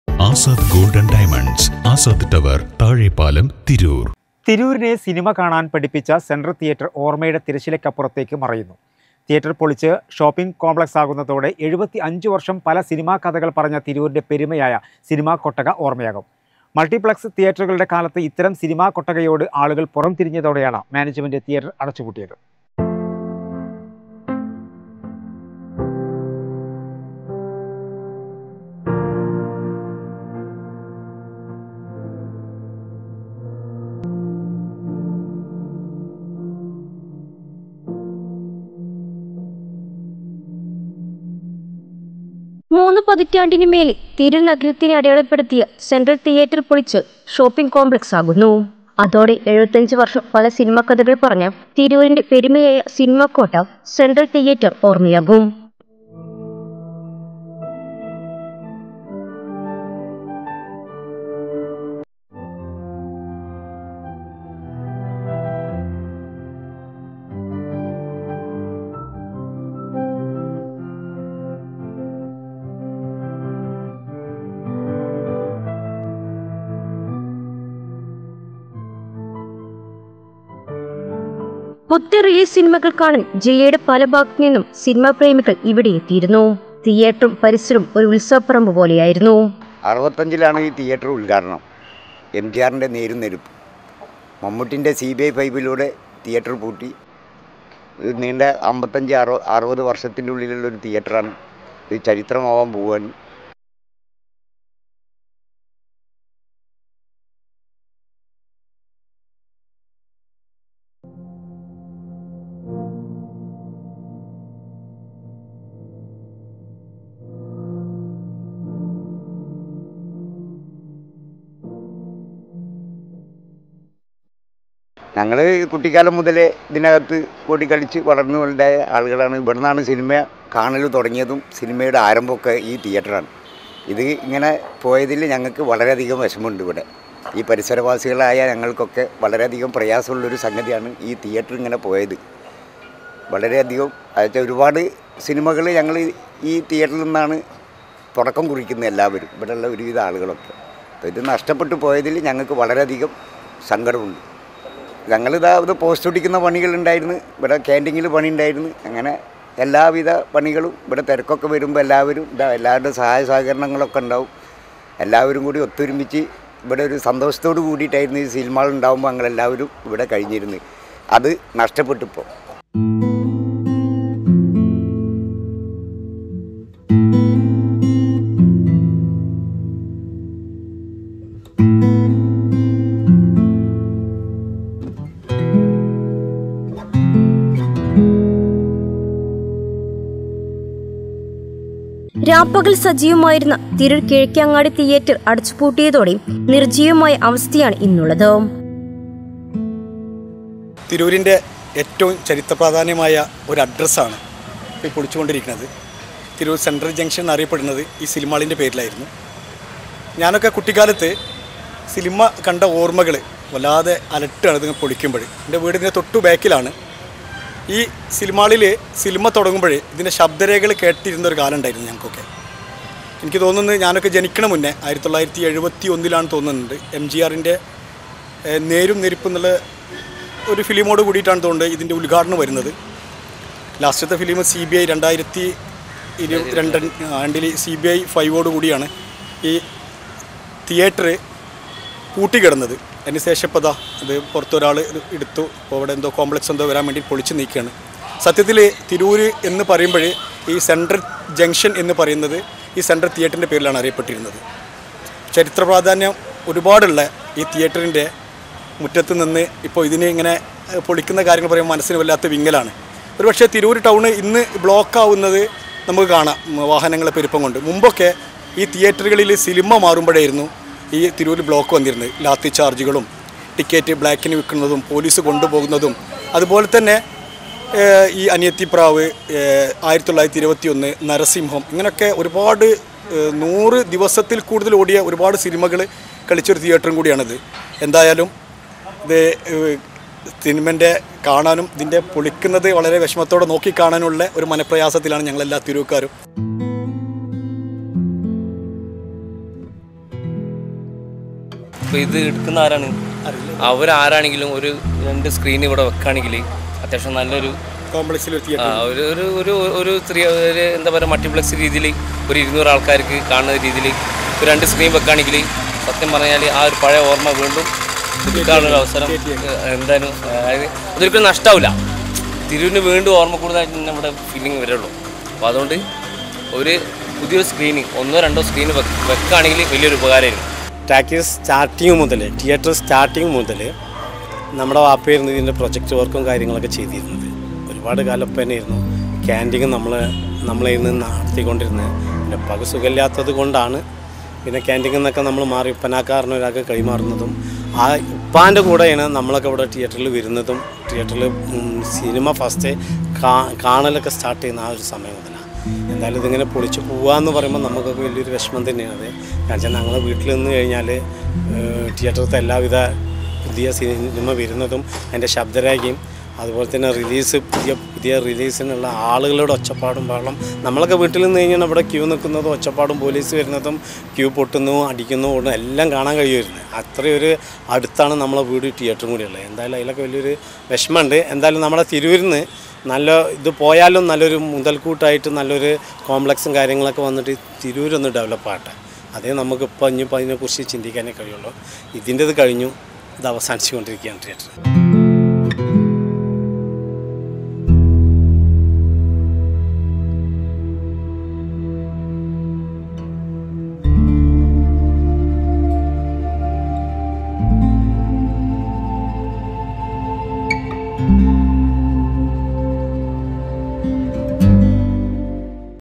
ആളും ആരവവുമൊഴിഞ്ഞു, തിരൂരിനെ സിനിമ കാണാന്‍ പഠിപ്പിച്ച സെൻട്രൽ തീയറ്റർ ഇനിയില്ല | Malayalam News
തിരൂരിനെ സിനിമ കാണാന്‍ പഠിപ്പിച്ച സെന്‍ട്രല്‍ തീയറ്റര്‍ ഓര്‍മയുടെ തിരശ്ശീലയ്ക്കപ്പുറത്തേക്ക് മറയുന്നു; തീയറ്റര്‍ പൊളിച്ച് ഷോപ്പിംഗ് കോംപ്ലക്സാകുന്നതോടെ 75 വര്‍ഷം പല സിനിമാക്കഥകള്‍ പറഞ്ഞ തിരൂരിന്റെ പെരുമയായ സിനിമ കൊട്ടക ഓര്‍മയാകും.